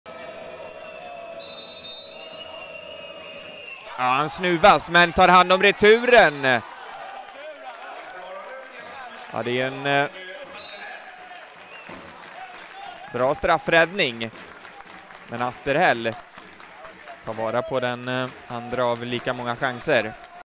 Hammarbys Hallberg nära att snuva Aranäs Atterhäll på målet - TV4 Sport
TV4 Sport från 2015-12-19: Grym  räddning av Hammarby Robin Hallberg som är nära att snuva Aranäs Tommy Atterhäll på målet.